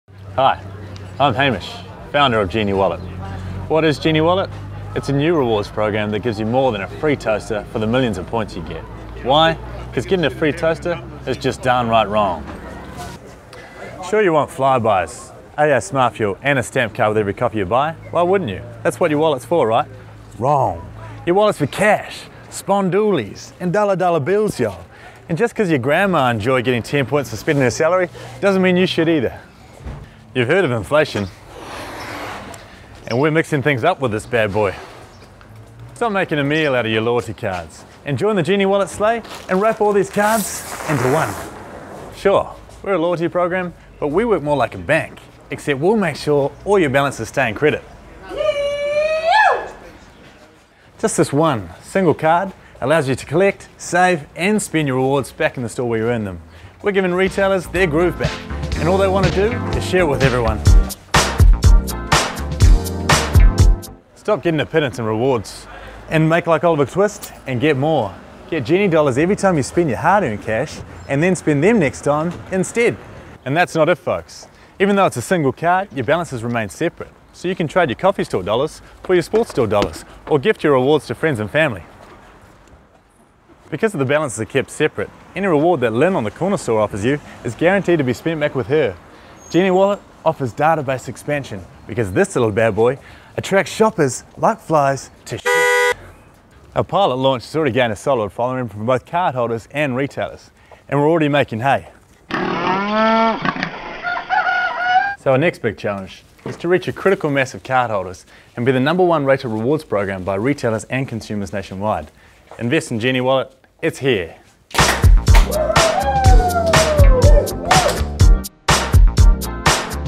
GenieWallet 2 Minute Pitch
New Zealand Rocket 2 Minute pitch created by Hamish Ludbrook.

GenieWallet.co.nz